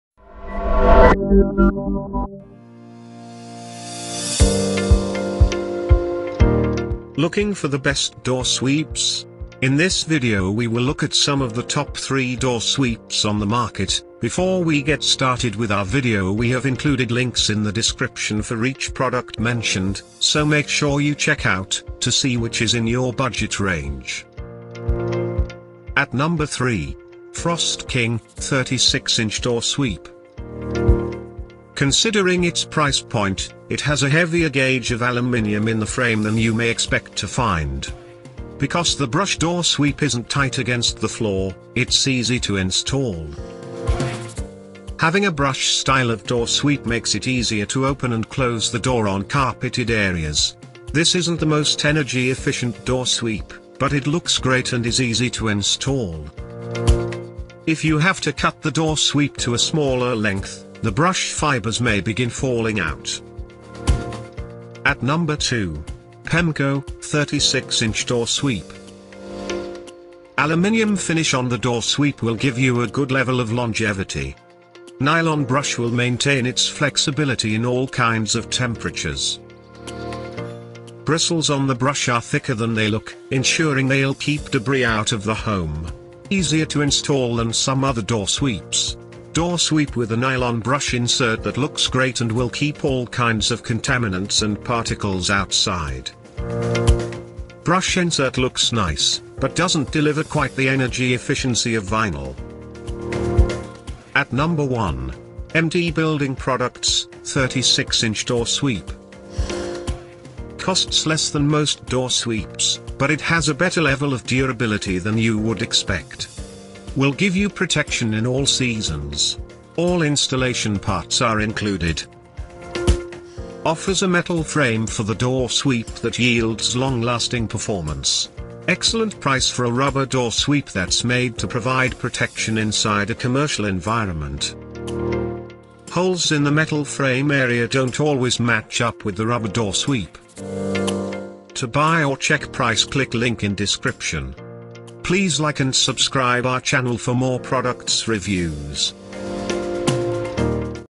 Best Door Sweeps, According To Customer Reviews in 2023
🌐 Best Door Sweeps

1. M-D Building Products 36-Inch Door Sweep.
BUoo

2. Pemko 36-Inch Door Sweep.
HRggEjw

3. Frost King 36-Inch Door Sweep
BDjY3Ao

0:00 Top 3 Best Door Sweeps
0:26 Frost King 36-Inch Door Sweep
1:04 Pemko 36-Inch Door Sweep.
1:44 M-D Building Products 36-Inch Door Sweep.